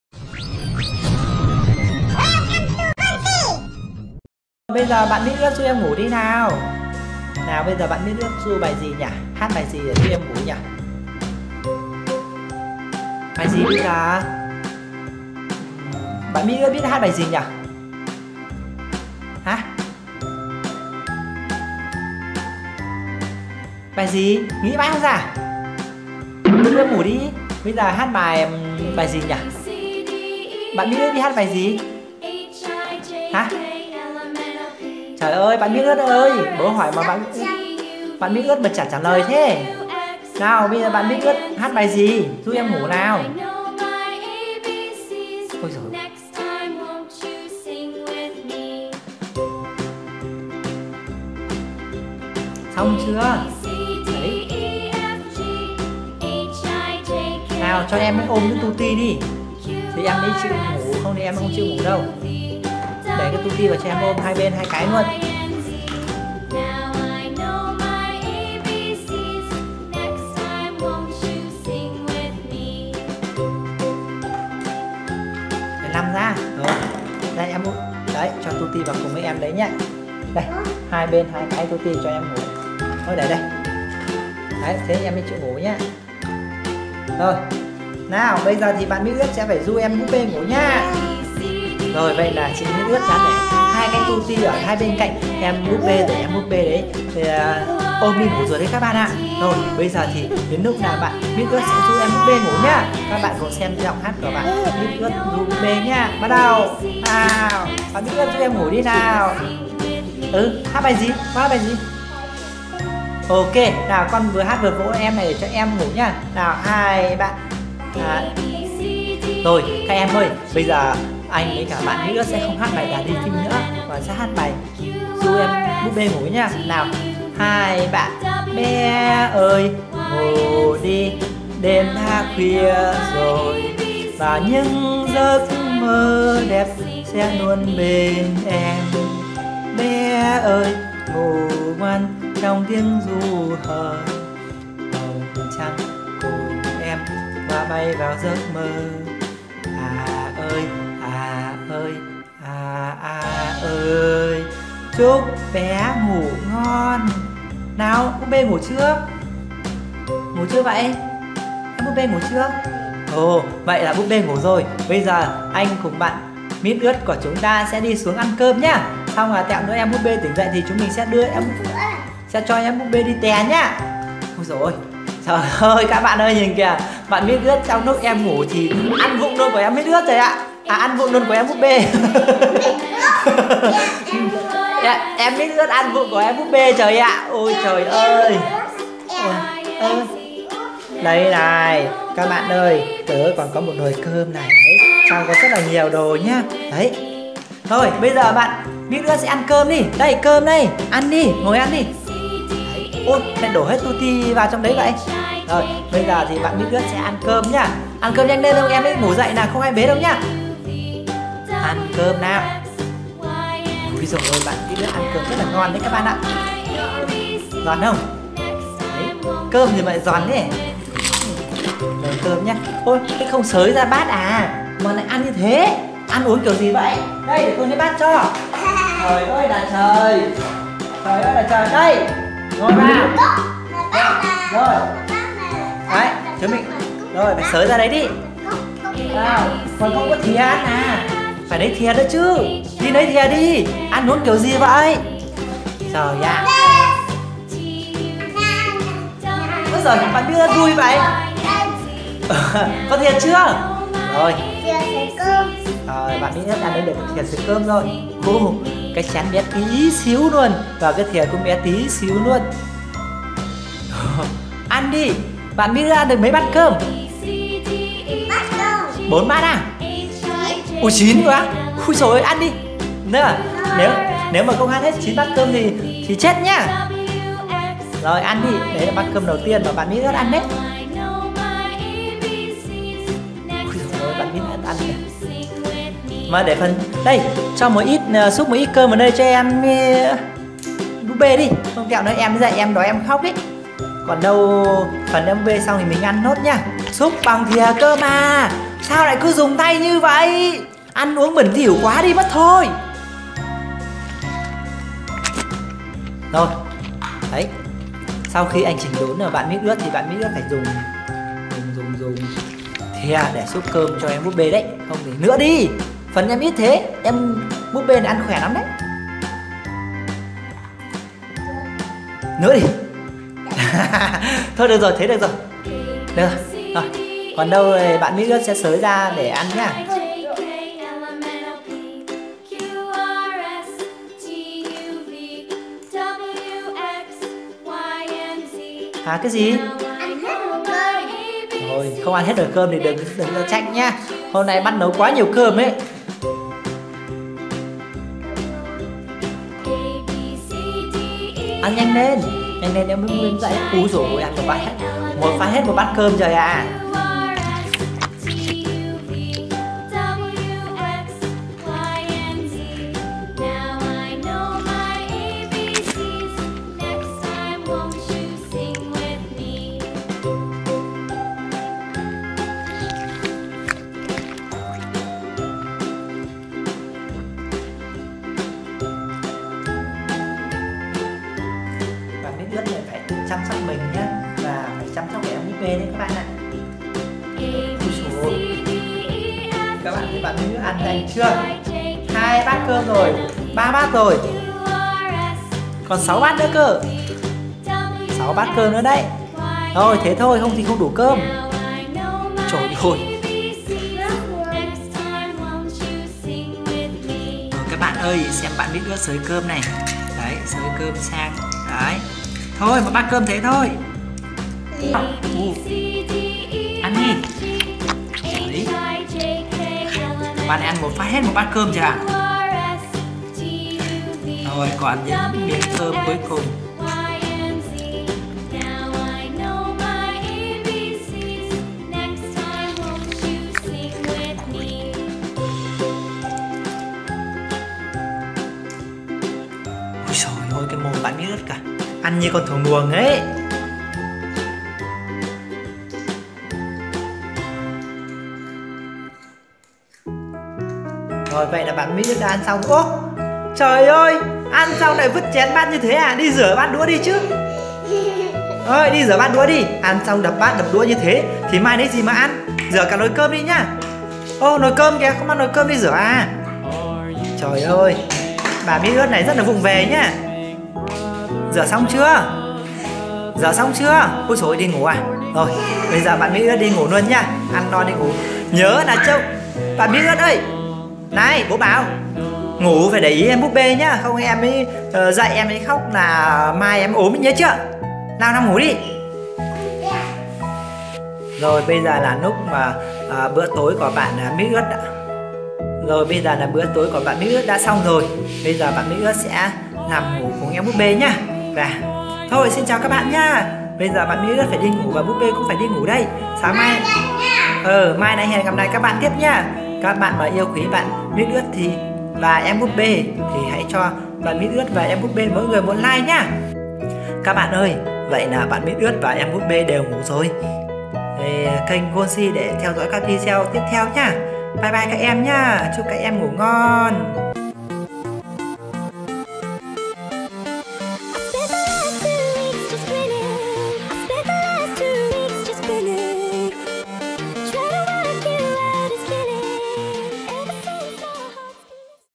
Mit Uot Và Đồ Chơi Trẻ Em Bath Tub [Phần 2] - Ru Búp Bê Ngủ
Sau khi tắm rửa song mit uot sẽ cho búp bê ăn rồi ru búp bê ngủ. búp bê ngủ thì mit uot ăn com và đi ngủ luôn các bạn ạ.
Chào mừng các em đến với Gold Sea